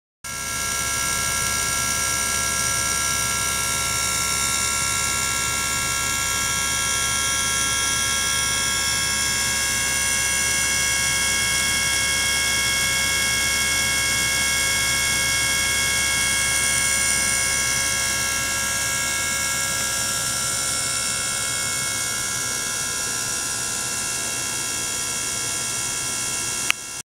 音圧測定解析に基づいた、「超音波シャワー」技術 Ultrasonic-labo
１００メガヘルツまでの周波数変化を含めた

ガラス・レンズ部品の精密洗浄
溶剤・洗剤・・・・の開発